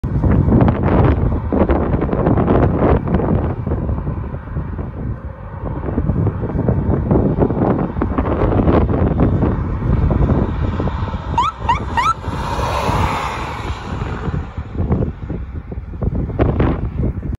Red Deer Emergency Services ALS ambulance Code 3
Red Deer ambulance 3371 enroute to the city with a patient from Clearwater County